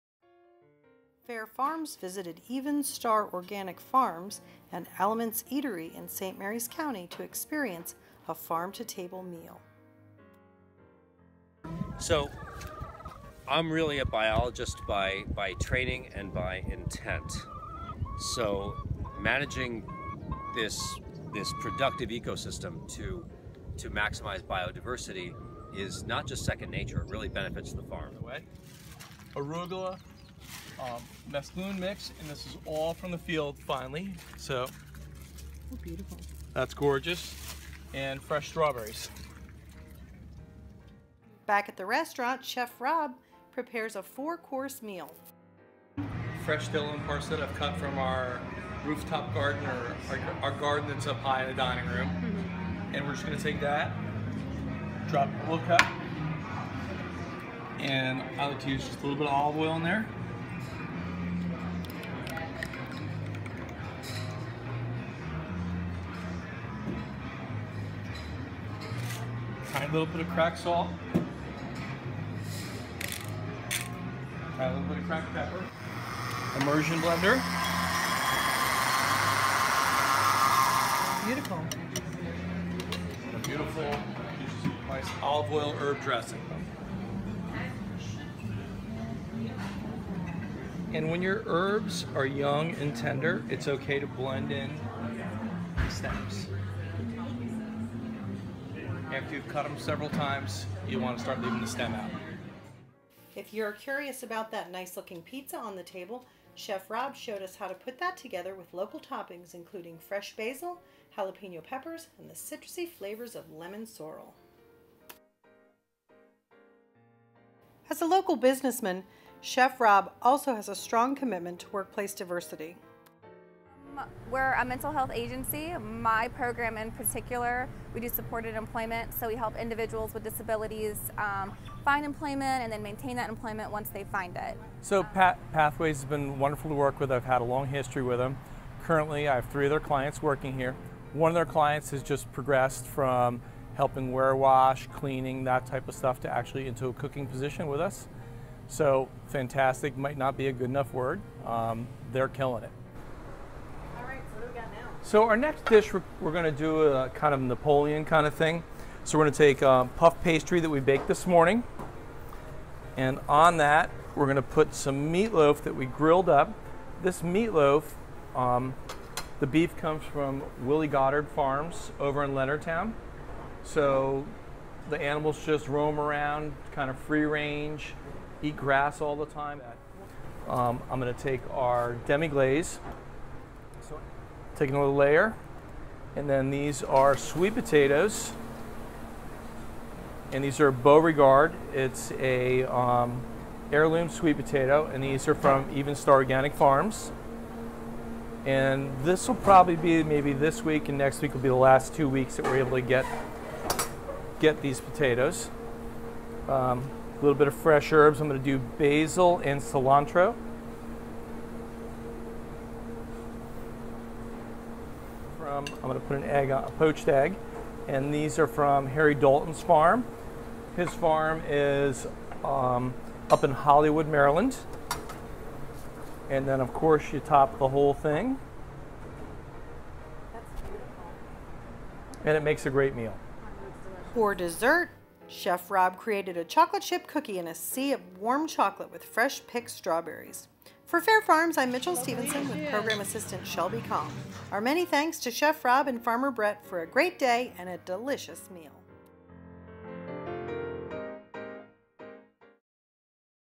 Farm to Table
Fair Farms visited Even' Star Organic Farm and Elements Eatery & Mixology, both in Lexington Park (St. Mary's County, MD), to see how a farm-to-table meal comes together. Join us on our journey from Farmer Brett's fields to Chef Rob's table.

for more information about the campaign.